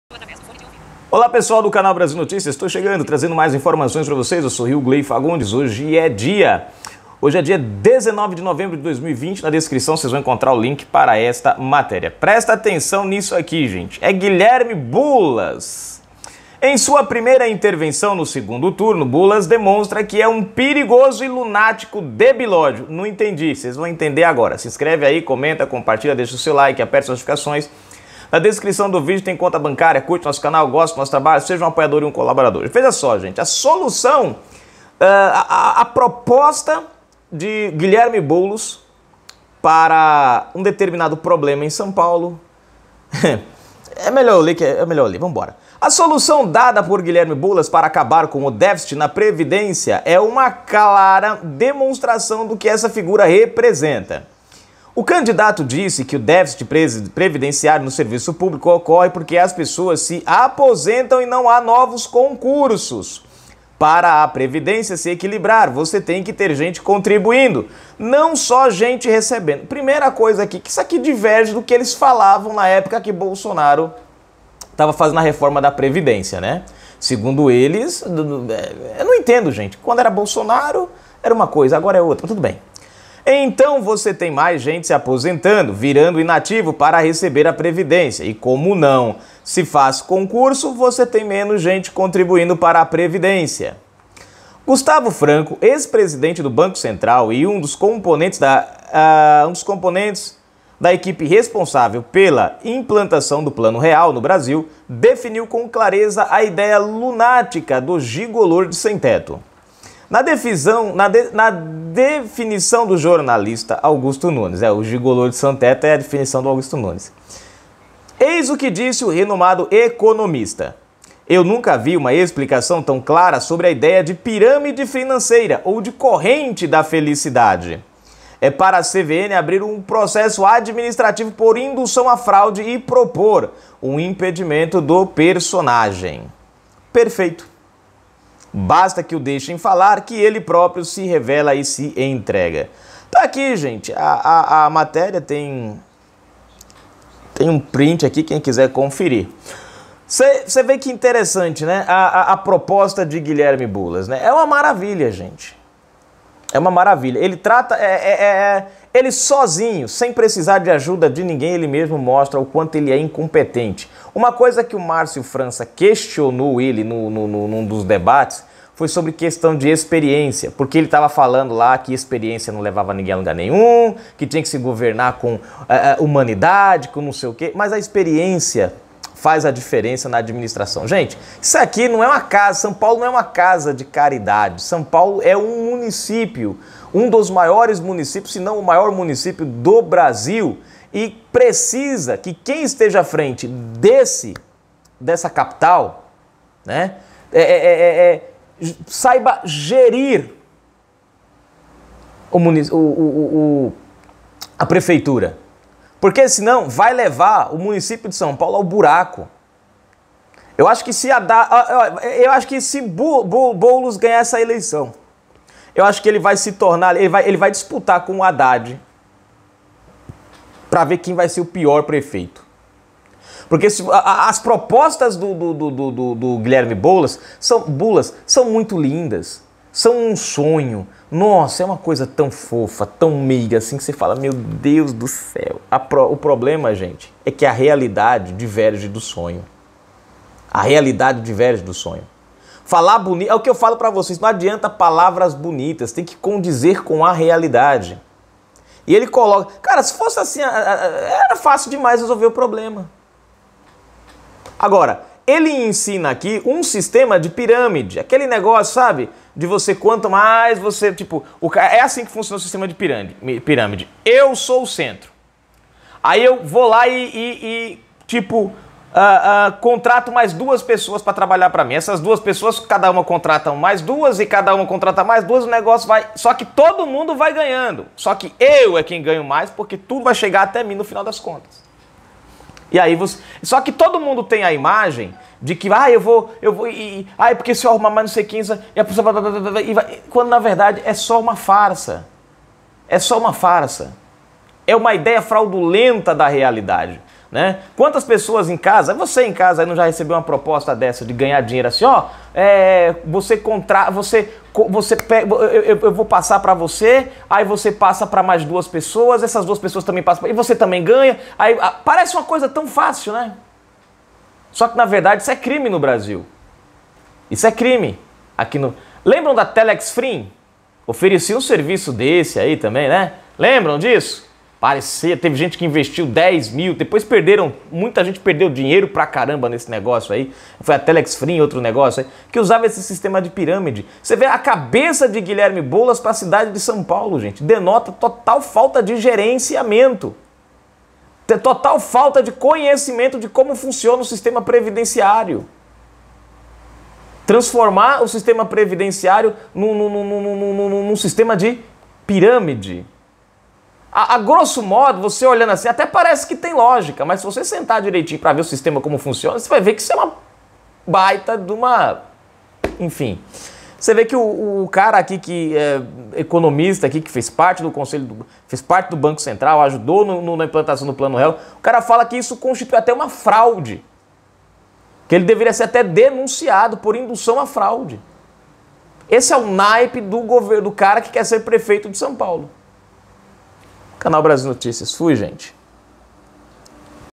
Proposta de Boulos prova incapacidade de gerência e induz a fraude previdenciária no sistema público
Ajude o canal, seja um colaborador:

Agencia 2121-0
Conta 5600-6
Variação 51
Nome Josefa V. Fagundes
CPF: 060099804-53